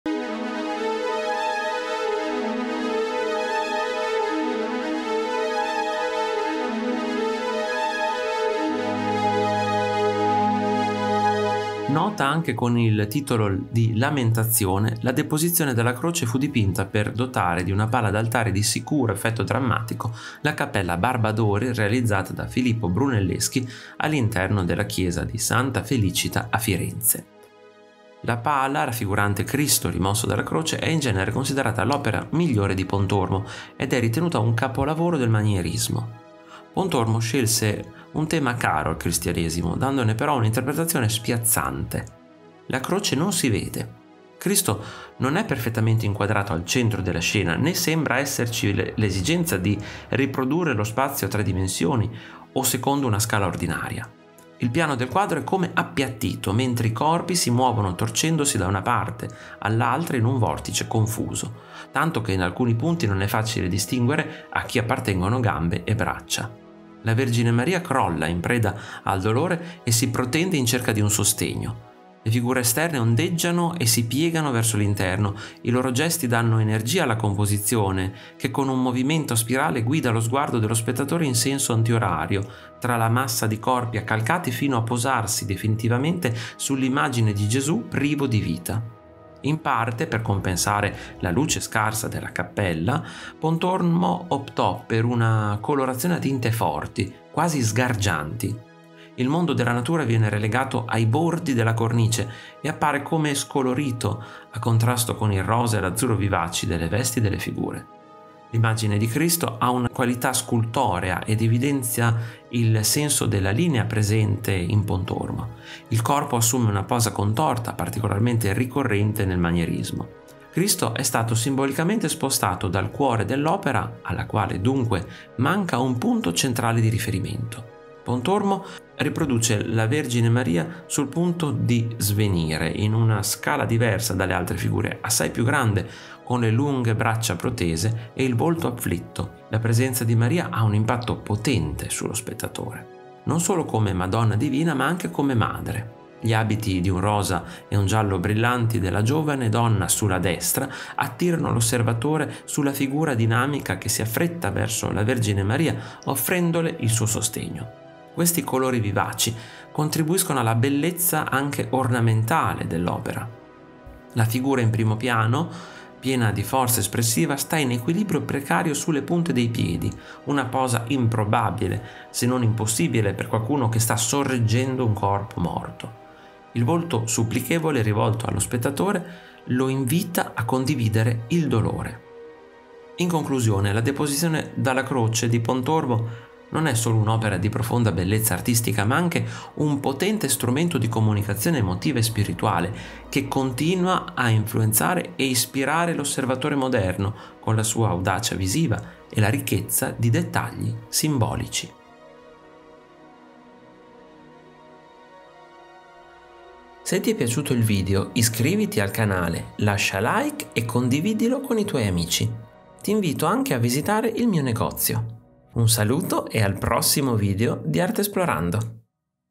Pontormo | La deposizione dalla Croce
L'audioquadro è un modo per conoscere i più grandi capolavori della storia dell'arte. In maniera semplice e in pochi minuti. Qui vi parlerò della a deposizione dalla Croce di Pontormo▼▼CONTINUA SOTTO▼▼

Pontormo, La deposizione dalla Croce, 1525-28

Nota anche con il titolo di Lamentazione, la deposizione dalla Croce fu dipinta per dotare di una pala d'altare di sicuro effetto drammatico la Cappella Barbadori realizzata da Filippo Brunelleschi all'interno della chiesa di Santa Felicita a Firenze. La pala raffigurante Cristo rimosso dalla croce è in genere considerata l'opera migliore di Pontormo ed è ritenuta un capolavoro del manierismo. Pontormo scelse un tema caro al cristianesimo, dandone però un'interpretazione spiazzante. La croce non si vede, Cristo non è perfettamente inquadrato al centro della scena né sembra esserci l'esigenza di riprodurre lo spazio a tre dimensioni o secondo una scala ordinaria. Il piano del quadro e come appiattito, mentre i corpi si muovono torcendosi da una parte all'altra in un vortice confuso.

LETTURE CONSIGLIATE